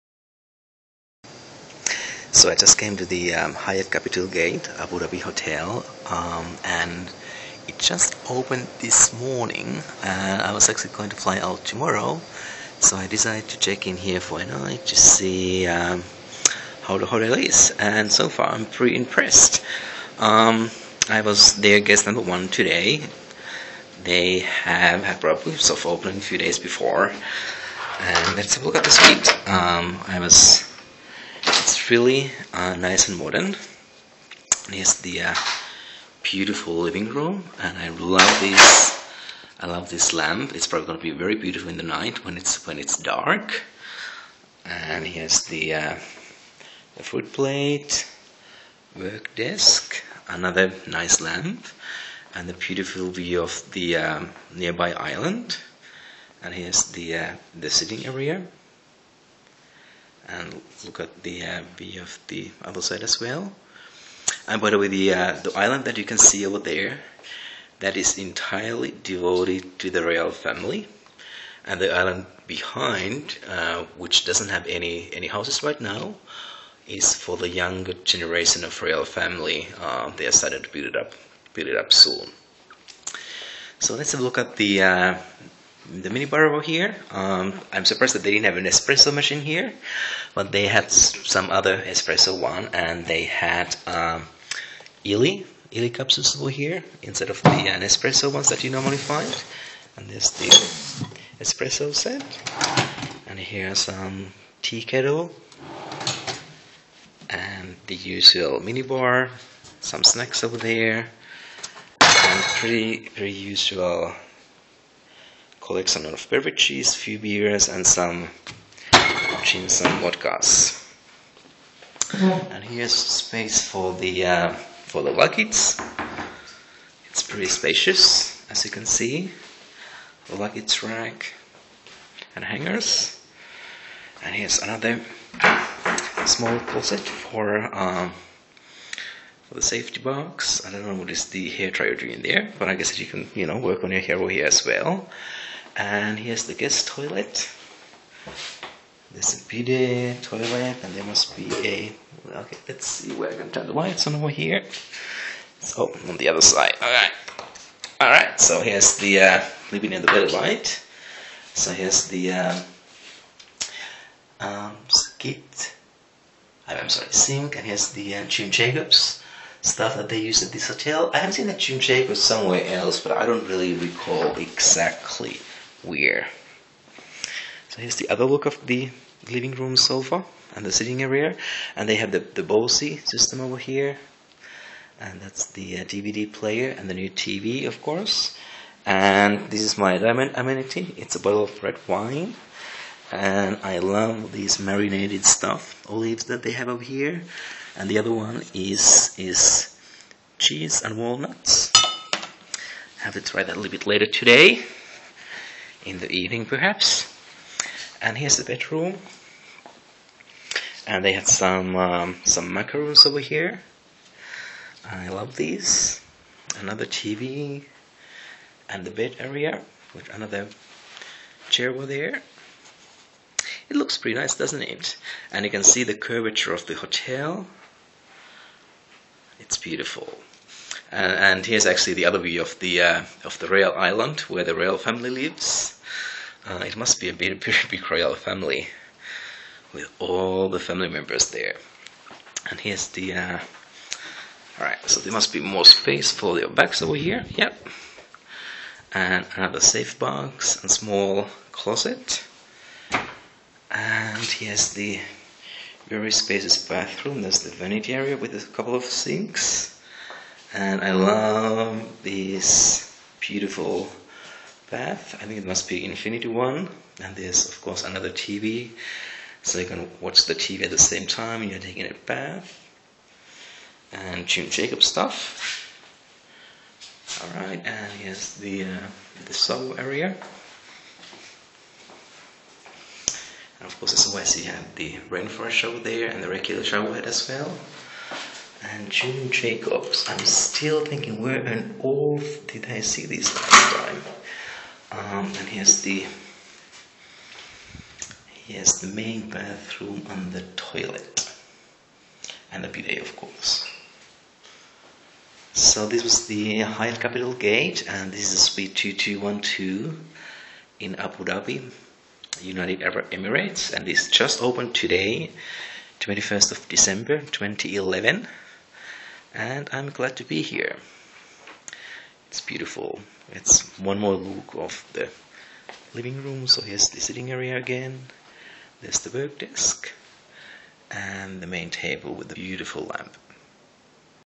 Hyatt Capital Gate, Abu Dhabi, UAE - Review of a Suite 2212
Review of a Suite 2212 at Hyatt Capital Gate, Abu Dhabi, UAE.
Check my review of the suite/hotel + more photos and read my travel blog